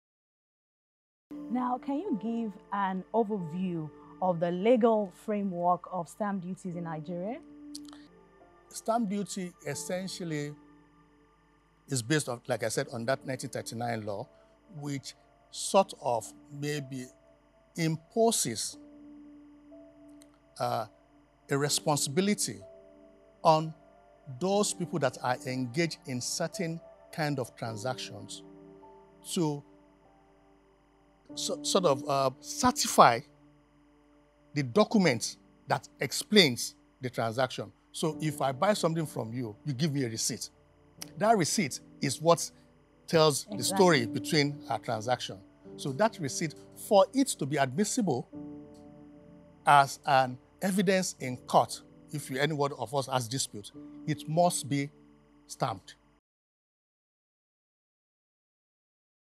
Stamp Duties and Taxation Strategies in Lagos State (Short Takes)
Stamp Duties and Taxation Strategies in Lagos State with Mr. Tokunbo Akande the Special Adviser to the Executive Chairman of LIRS.

Facebook: Lagos State Internal Revenue Service - LIRS